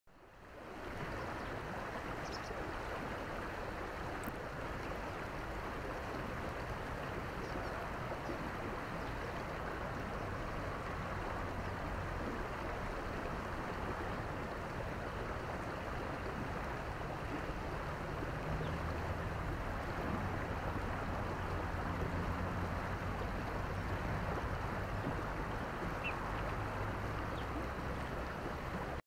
Ruddy Turnstone
Ruddy Turnstone, Arenaria interpres - Discover the life of birds in High Definition.
Recorded in Penzance, United Kingdom.

Simply Stunning Birds is dedicated to photographing wild birds from across the globe in their natural habitats.